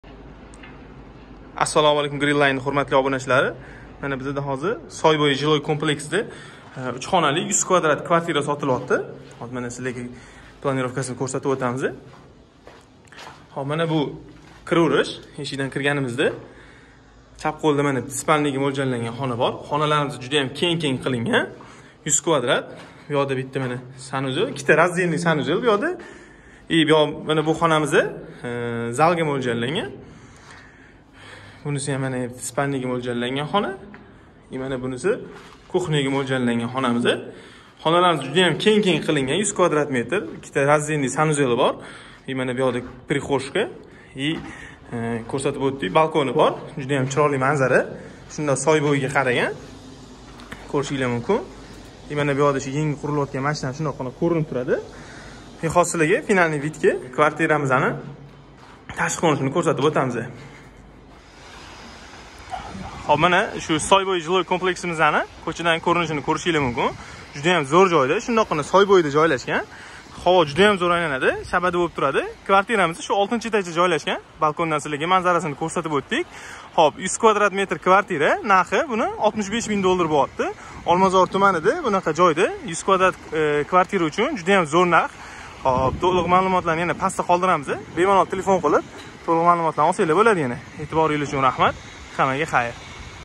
SOTILDI❗️❗️❗️🤩 100kv! Olmazor Tumani! 3 xona! Soy bo’yida! 🤩квартира (Коробка) greenlineuz
➖Продаётся : Квартира
📞SOTILDI❗️
➖Общая площадь: 100м²
➖Тип строения: Кирпичное
➖Количество комнат: 3
➖Санузел: 2
➖Этажность: 9
➖Этаж: 6
➖Описание: Форма оплаты Любая❗️❗️
➖ Адрес : Алмазарский р-н, ЖК. "Soy Bo'yi"
➖ Цена : 65,000$
➖Подробно:📞SOTILDI❗️❗️